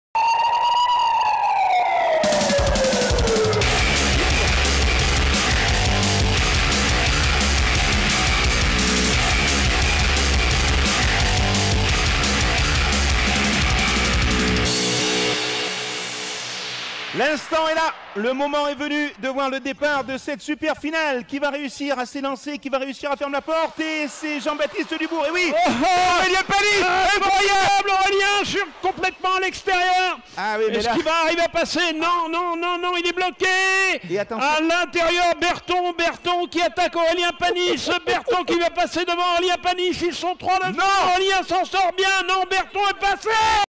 Dépassement Berthon | Isola 2000 | e-Trophé Andros 2020
Course 1 | Isola 2000 | Super Finale ELITE PRO |

Les deux ZOE étaient placées en 1ère ligne mais dès le départ Nicolas Prost n’a pas pu résister à l’attaque de Nathanaël Berthon - #5 Peugeot 208 (Sylvain Pussier Compétition) totalement déchaîné et s’est retrouvé en bout de peloton juste devant Olivier Panis - #4 Petit Forestier AS 01 (Sébastien Loeb Racing) qui fermait la marche.